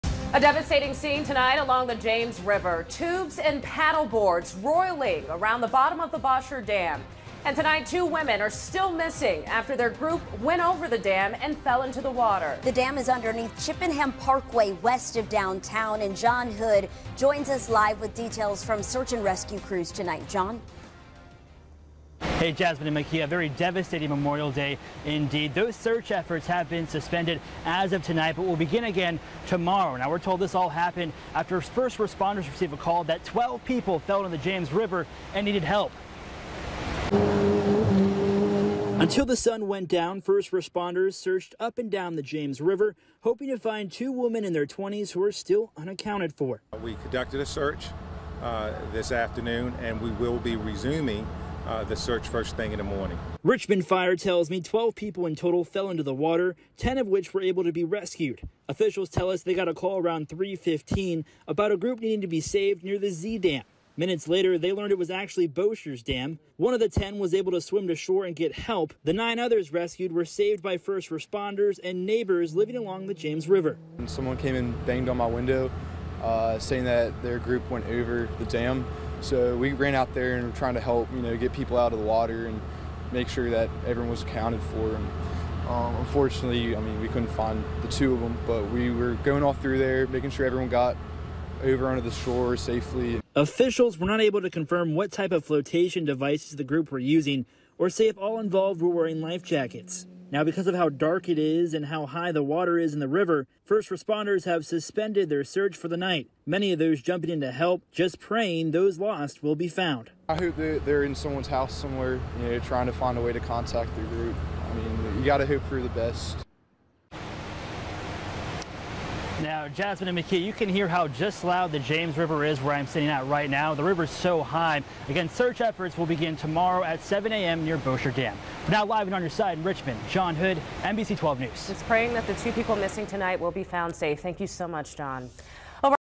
Crews search James River for 2 missing people
Richmond crews are searching for two missing people after going over the Bosher Dam on the James River.

Officials said 12 people were involved in the incident at Bosher Dam, which happened around 3:15 p.m. on Memorial Day. FULL STORY